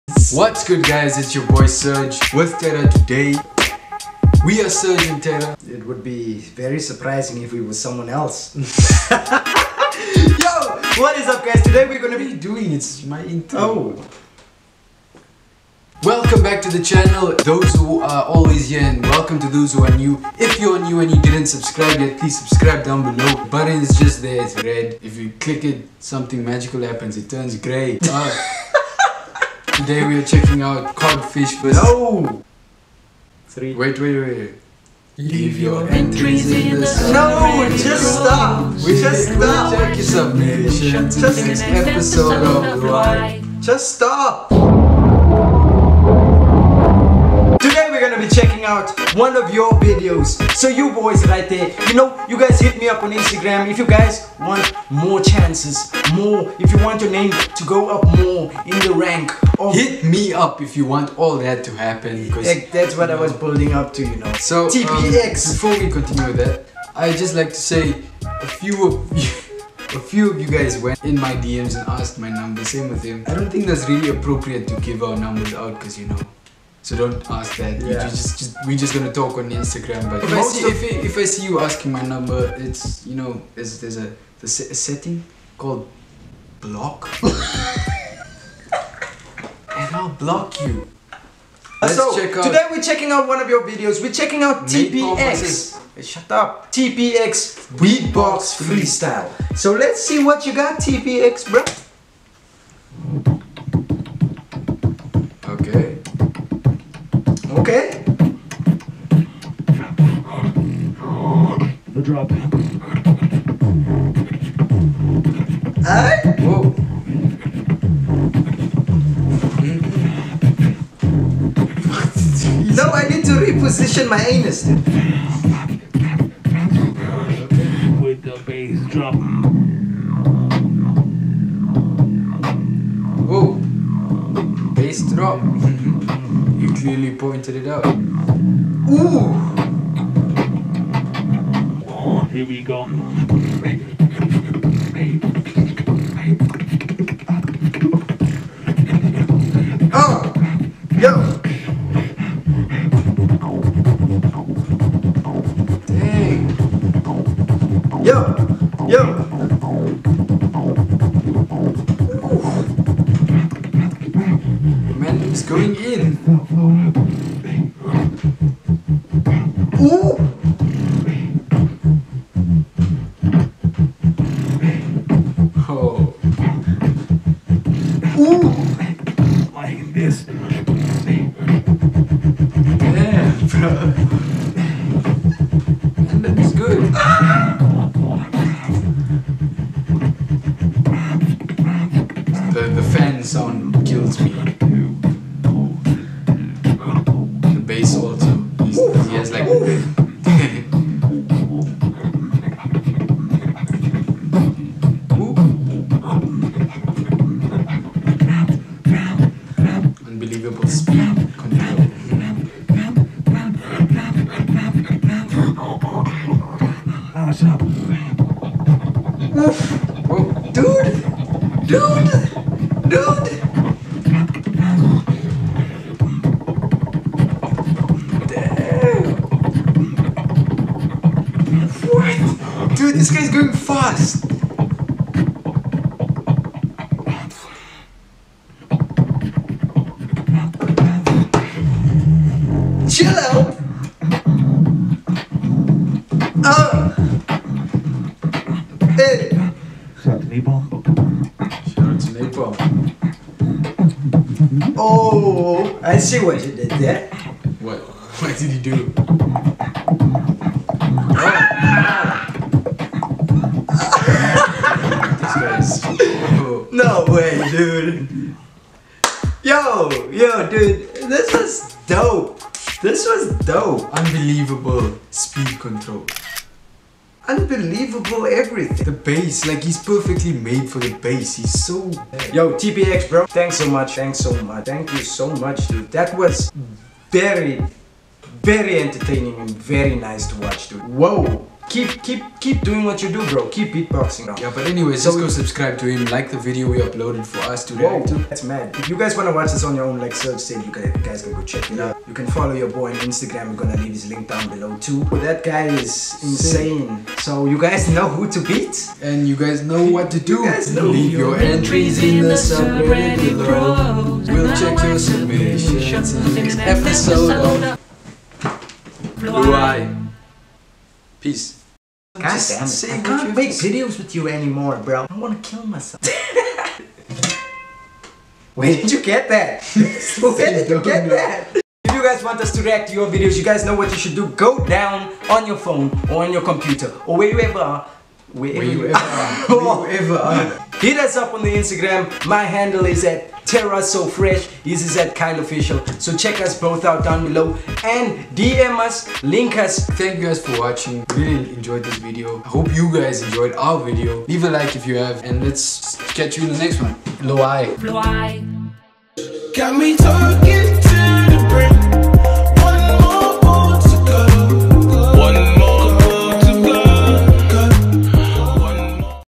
TPX Beatbox Freestyle || REACTING TO YOU GUYS BEATBOXING
Guys don't forget to hit us up on Instagram if you want us to check out your beatboxing videos. Thank you to TPX for sending us his video, he really smashed it and we enjoyed it so much.

Post you comments Down Below on what you want us to react to next.

Video Uploaded By:
SyrGe/Terra/SyrGe&Terra/S&T